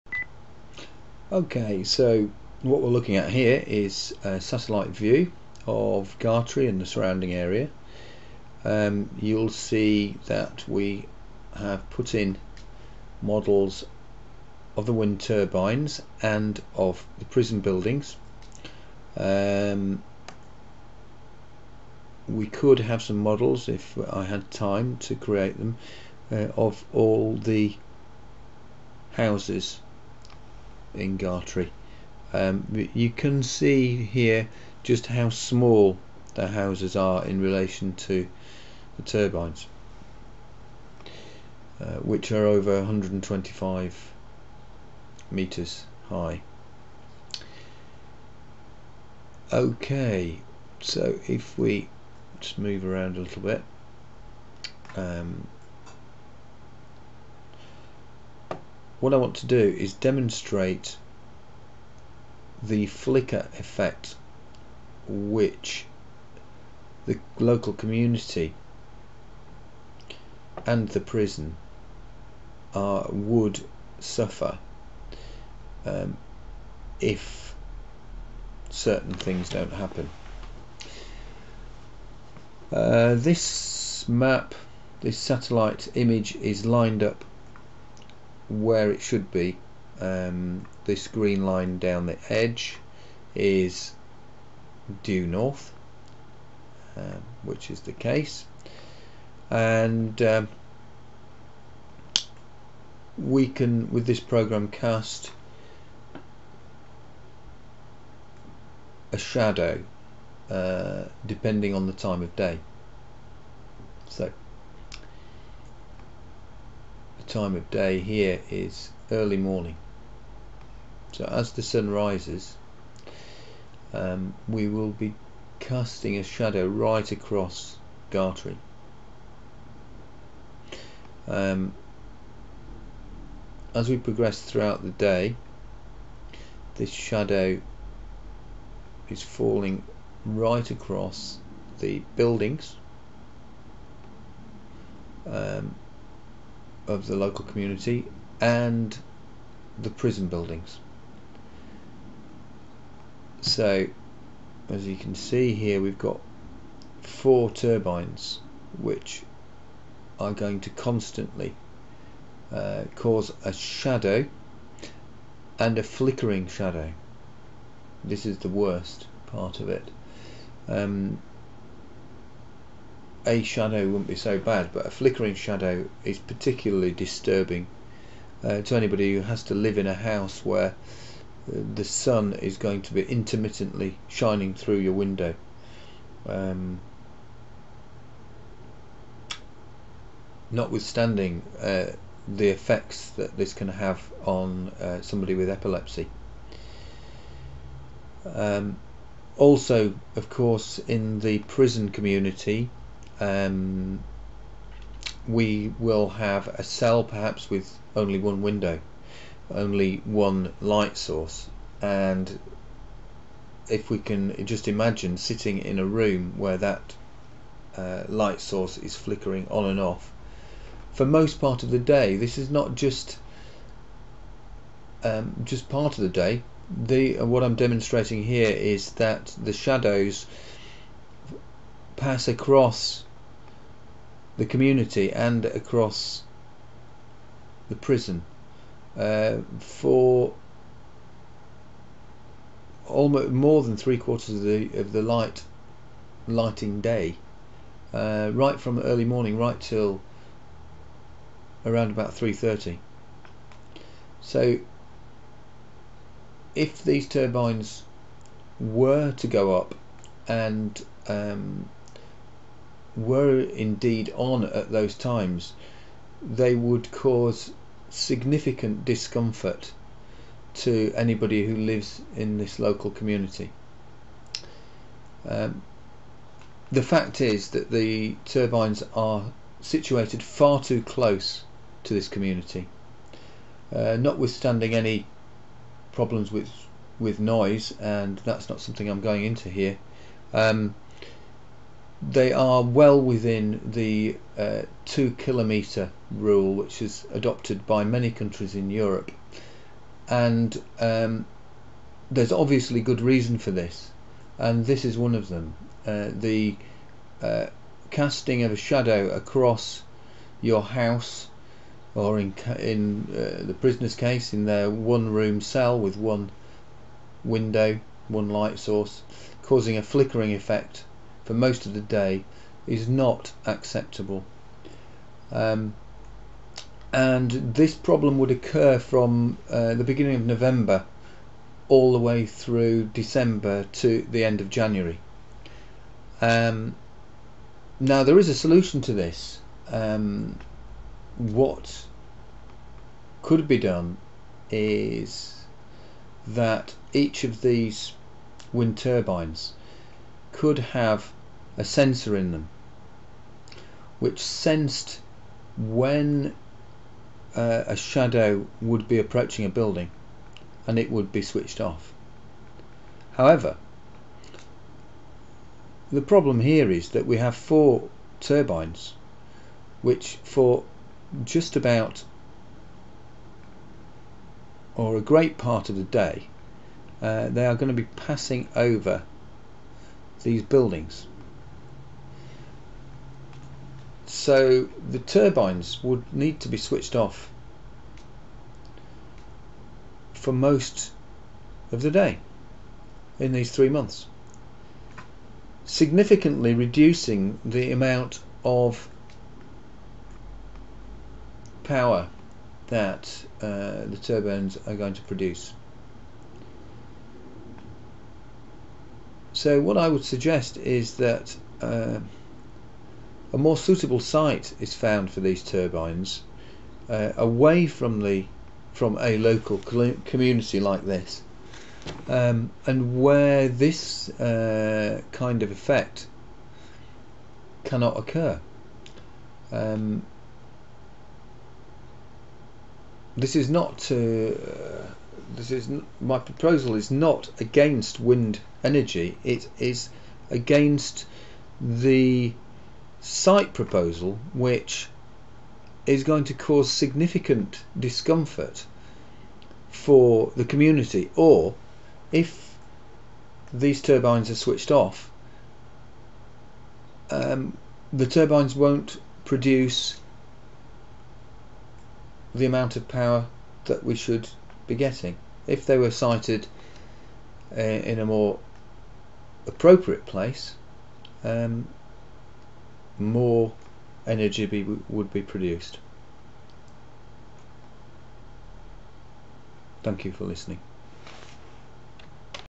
Wind Turbine Shadow Flicker Impact
The shadow flicker impact on buildings within the vicinity of the  wind farm is going to be significant. These buildings include the prison at Gartree where flickering light through a single window light source is going to be very disturbing.
Sensors can be used to turn off the turbines when shadows cross over buildings but this would mean that these turbines would not be turning for a significant amount of time per year (i.e. between beginning of November and end of January) and would therefore not be efficient. Surely a more suitable site away from buildings can be found.